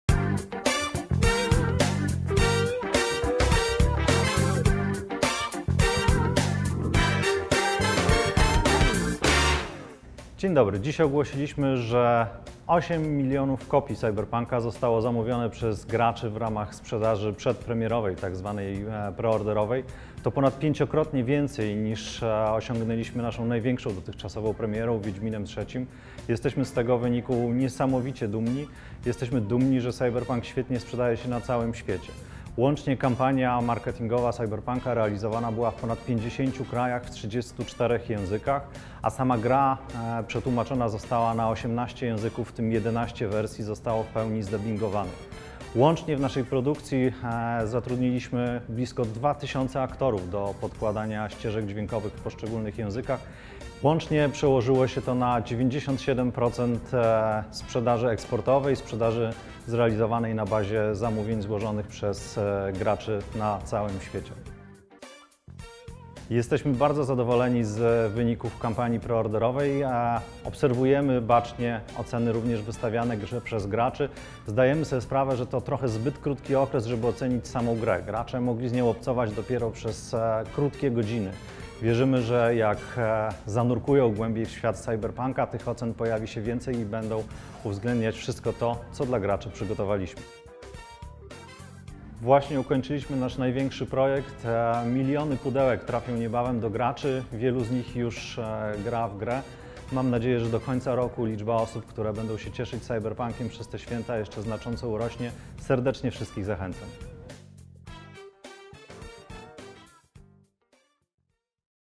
CD PROJEKT SA, Piotr Nielubowicz – Wiceprezes Zarządu ds. Finansowych, #386 ZE SPÓŁEK
O największej i najbardziej wyczekiwanej produkcji ostatnich lat opowiada Piotr Nielubowicz, Wiceprezes Zarządu ds. Finansowych, CD PROJEKT SA.